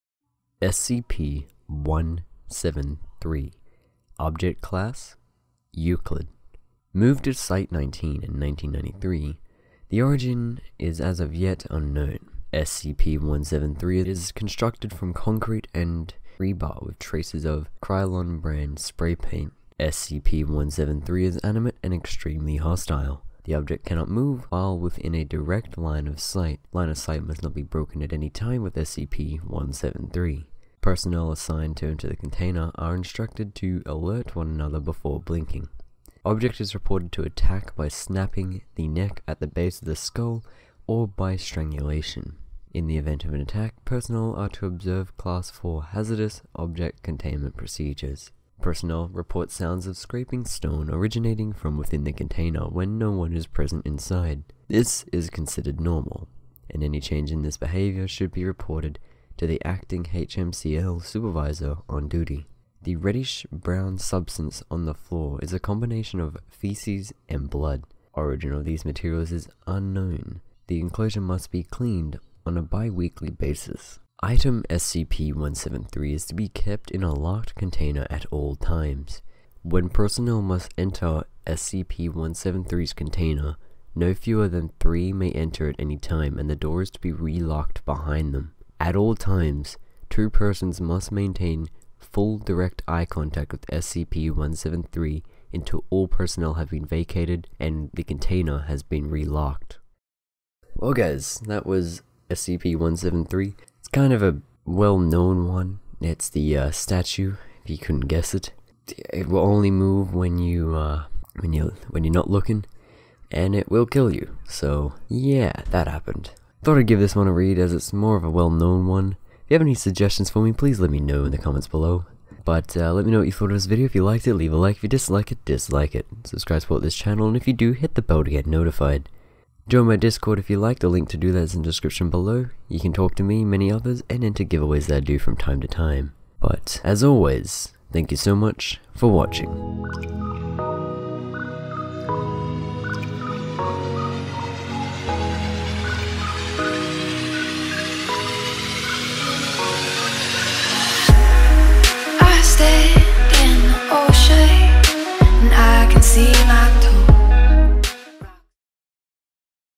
SCP - 173
Today i read out scp-173's documentation. A animate statue that proves to be lethal.

Social Network Accounts